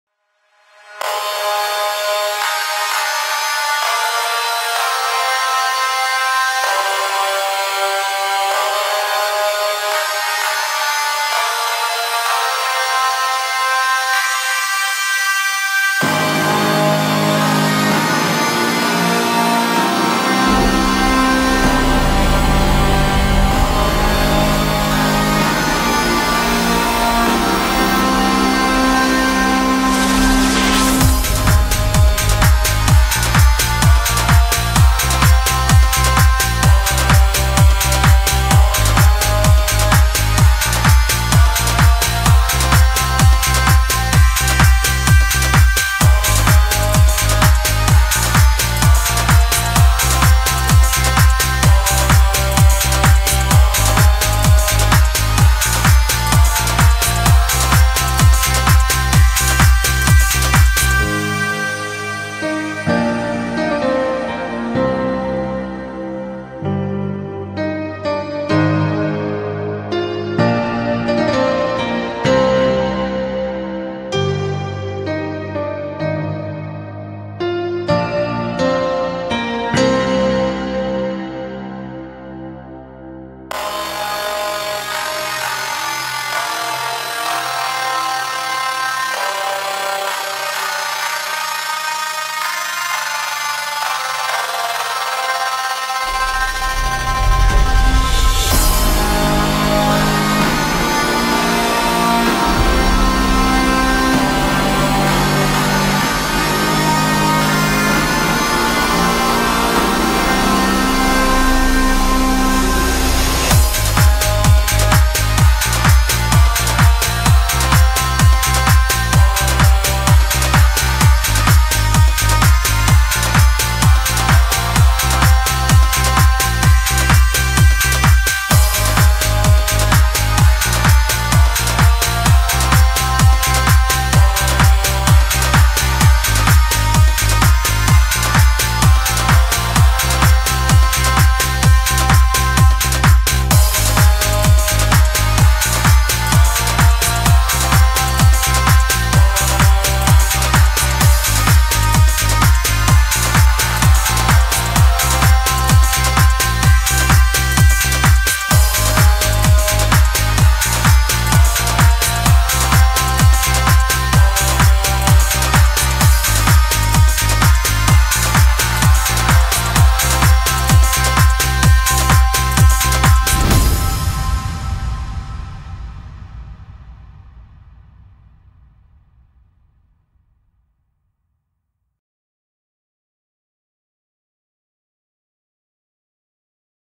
Catchy Upbeat Background Music
Catchy upbeat background music that you can use for workout, study, or as background music in videos, games etc.